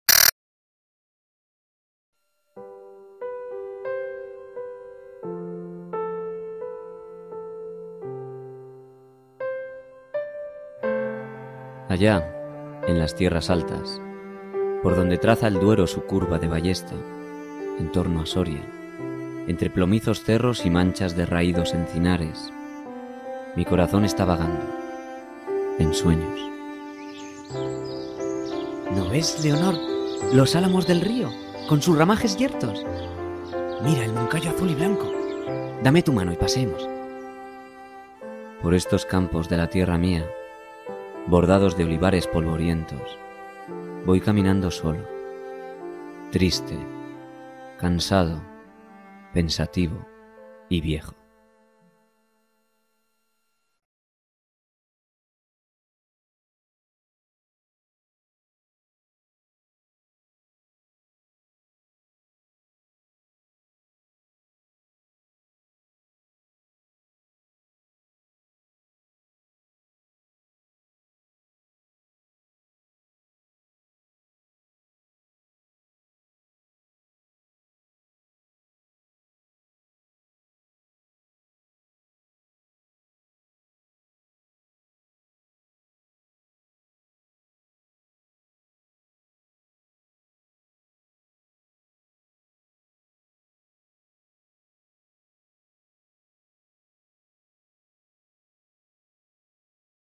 Allá en las tierras altas
"Allá en las tierras altas", de Antonio Machado.
Voz: Jorge Da Rocha. Tema musical:Javier Moreno.